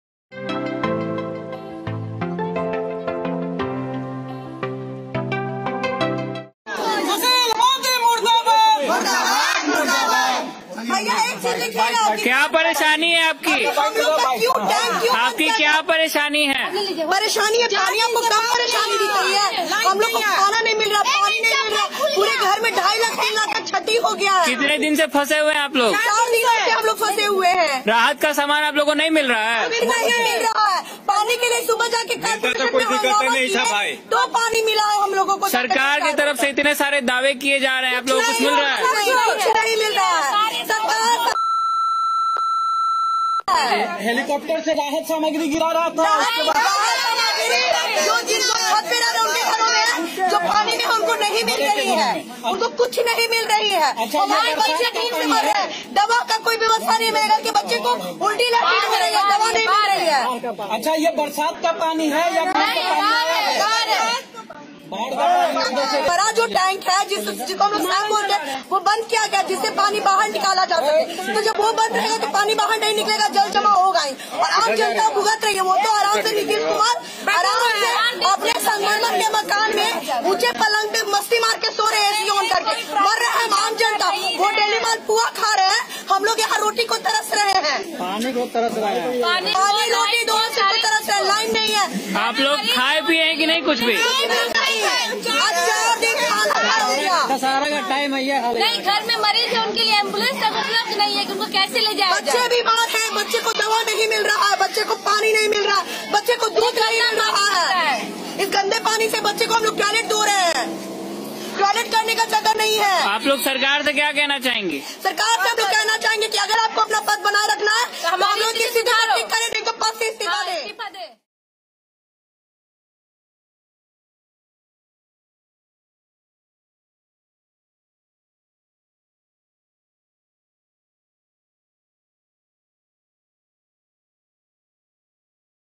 Patna: बाढ़ में परेशान लोगों ने CM Nitish Kumar से मांगा इस्तीफा
पटना में हुई लगातार बारिश से लोग हैं परेशान... परेशान लोगों ने पानी में खड़े होकर सरकार का किया विरोध... लोगों ने सीएम नीतीश कुमार के खिलाफ लगाए नारे... लोगों ने सीएम से मांगा इस्तीफा...